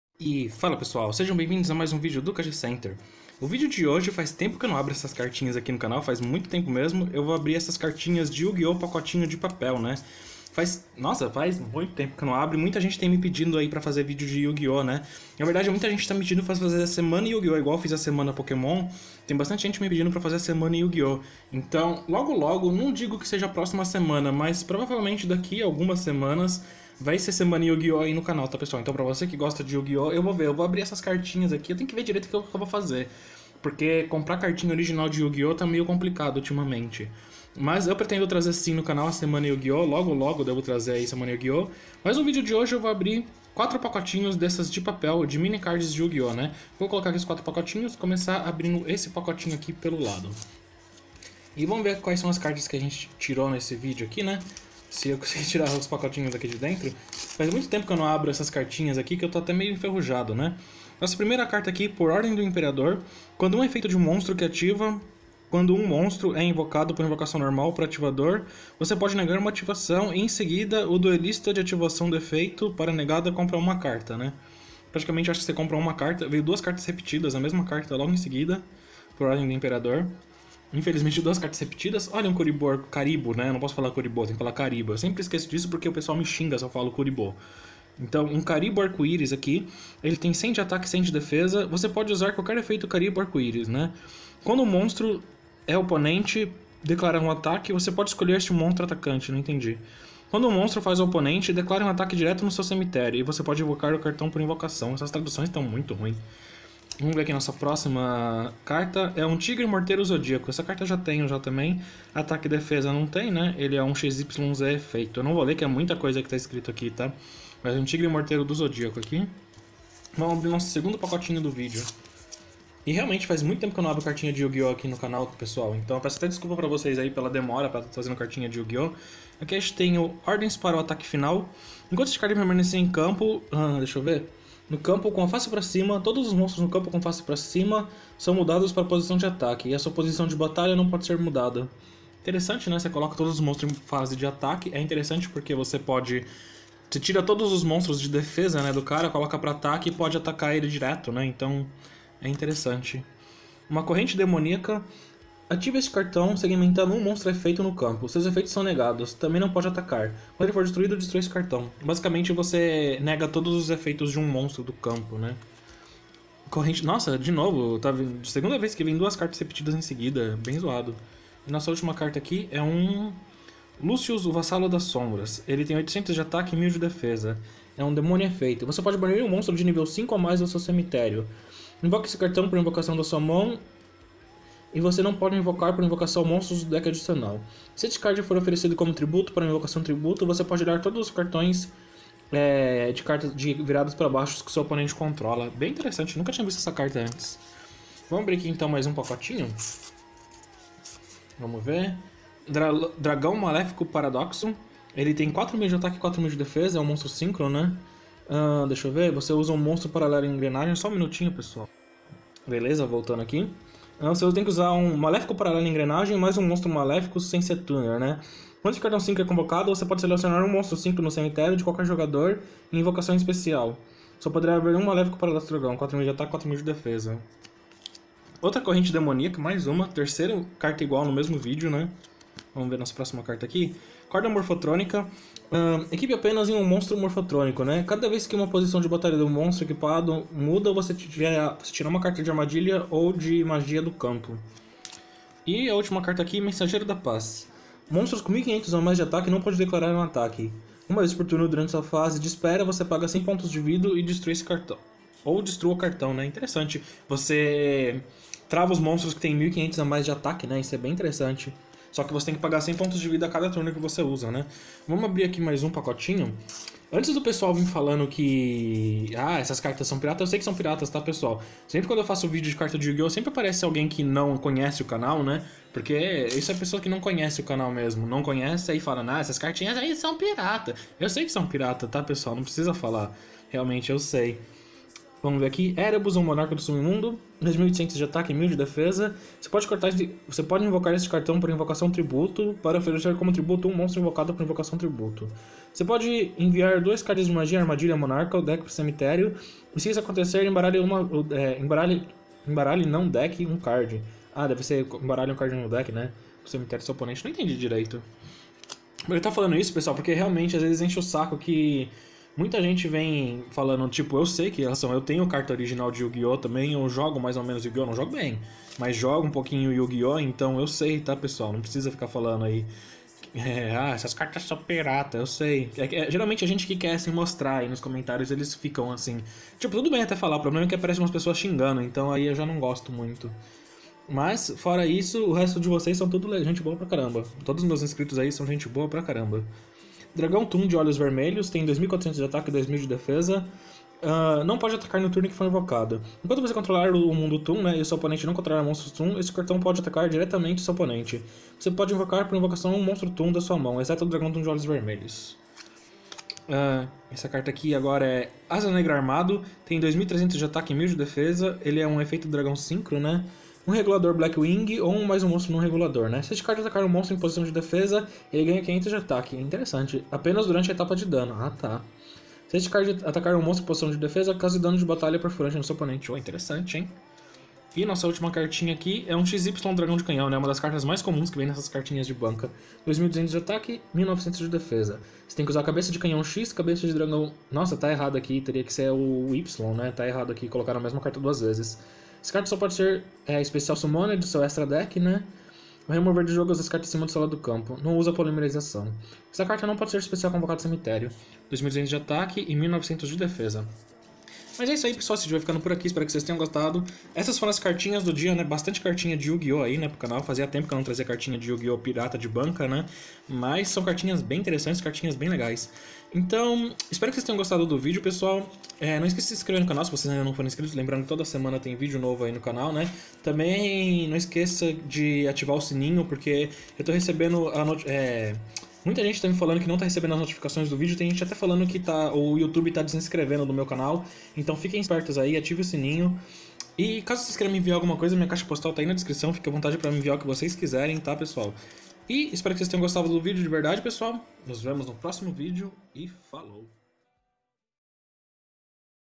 E depois de muito tempo voltamos com Cards de Yu-Gi-Oh!
Fala pessoal, sejam bem vindos a mais um vídeo do KG Center, no vídeo de hoje estamos abrindo cards de Yu-Gi-Oh.
ME ENVIE ALGO

FELIPE REIS LEANDRO BERTOLDO
Caixa Postal 2009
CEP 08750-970
Mogi das Cruzes - SP
CANAIS PARCEIROS!

Tamo Junto pessoal ;)